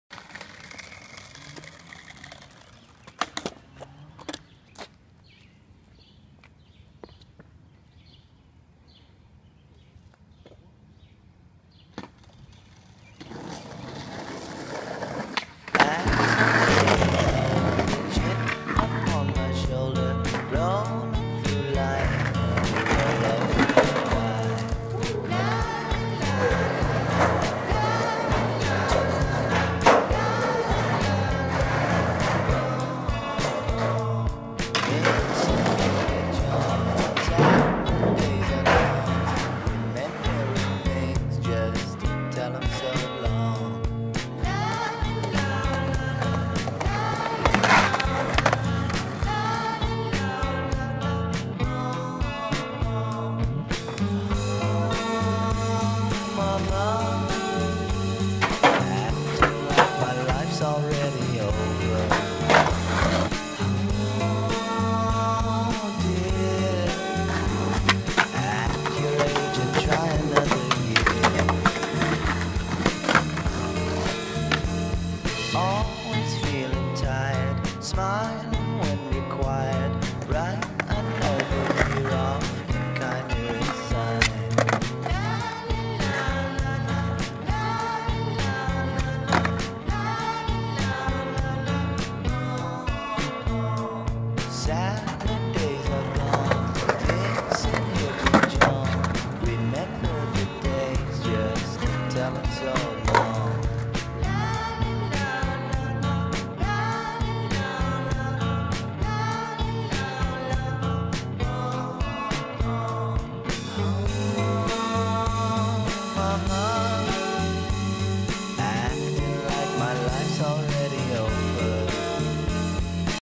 Blueberries
We try to make a video in the style of fat bill. Skateboarding from Richland Center, Wisconsin.

Gavin McCauley

Daniel Johll